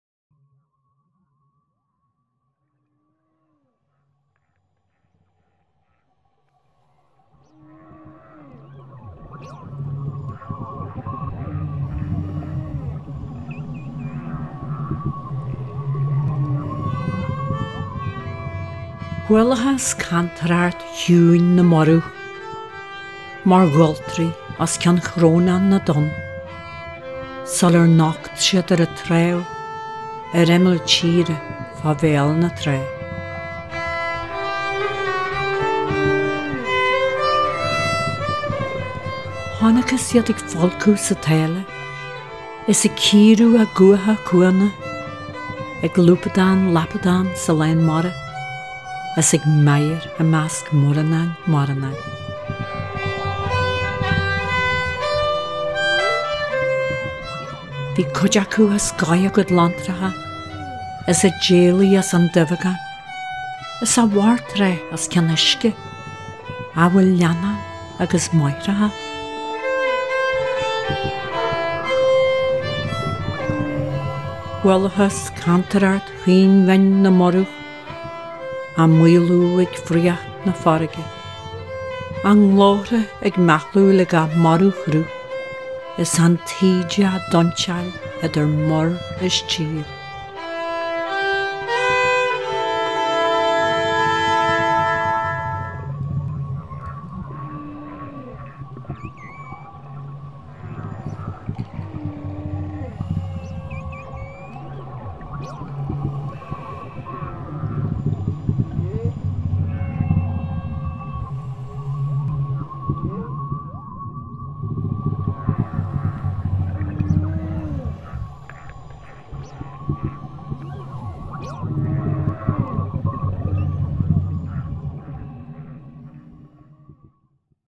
Seachtain na Gaeilge - Léamh Filíochta le Máire Dinny Wren - 14 Márta 8:00 i.n
Tá ‘Cantaireacht na Murúch’ a bheas a rá ag an fhile Maire Dinny Wren, ar cheann de na dánta is áille
sa chnuasach Tine Ghealáin.

Poetry Session - Maire Dinny Wren
The poet Maire Dinny Wren recites “Cantaireacht na Múruch” from her latest published book of poetry Tíne Ghealáin.